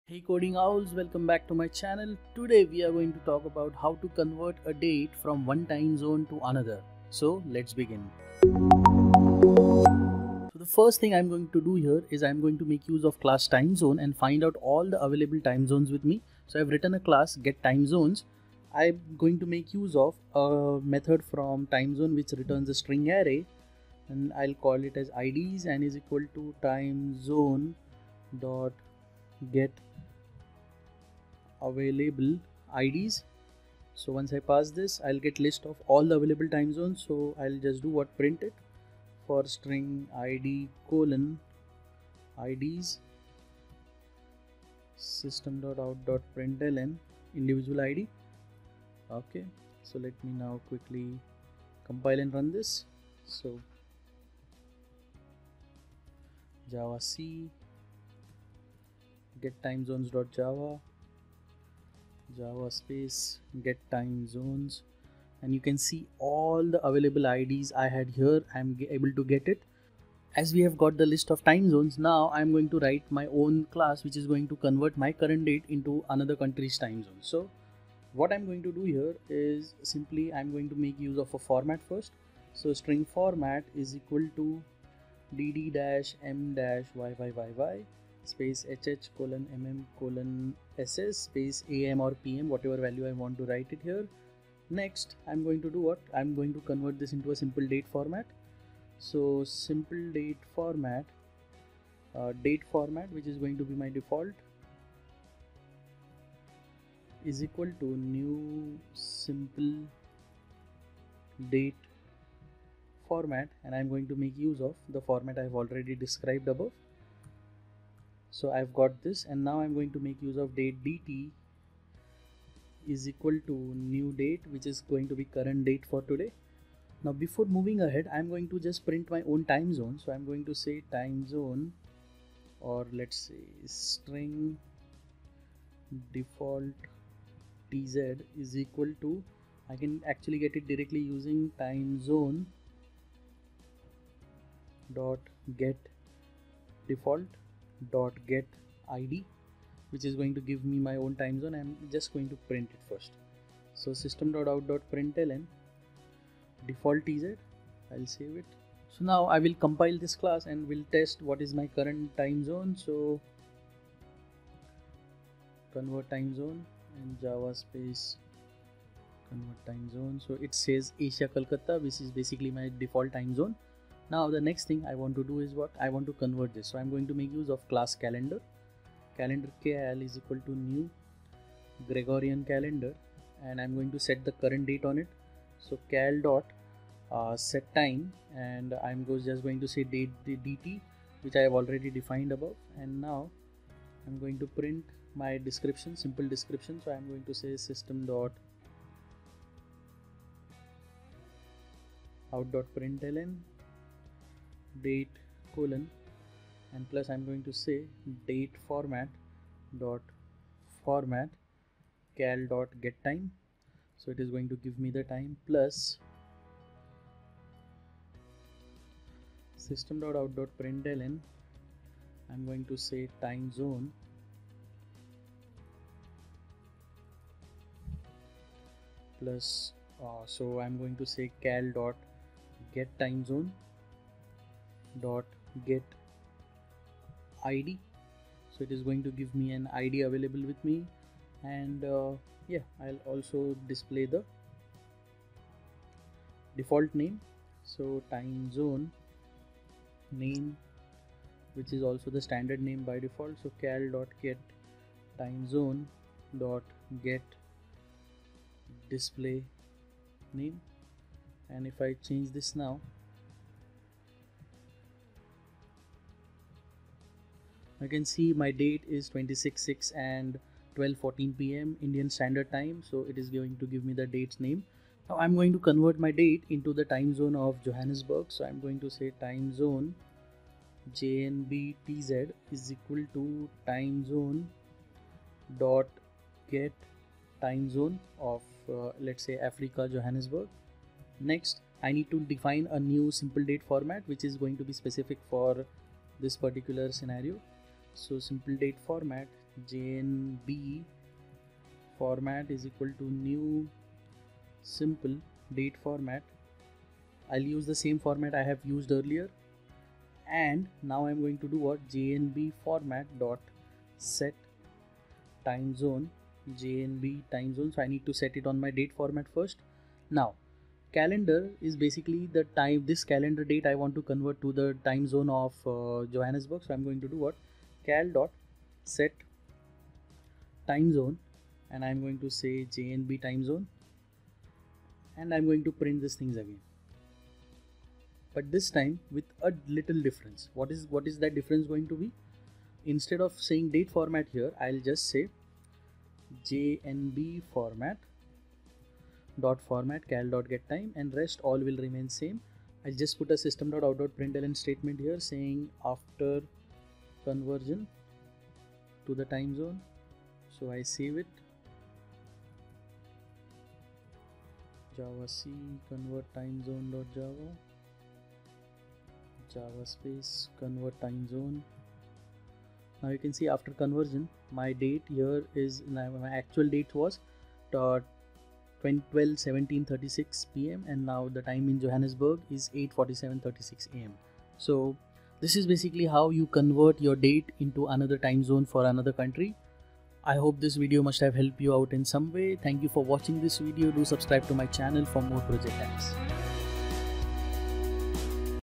Converting Timezone of a Java Calendar Object
Hey Coding Owls!!

This video I have demonstrated how to convert a timezone for a Calendar object. Let say you are working on a project with multiple countries involved, so you might need to convert your current date and time to another timezone. This video will help you to achieve that.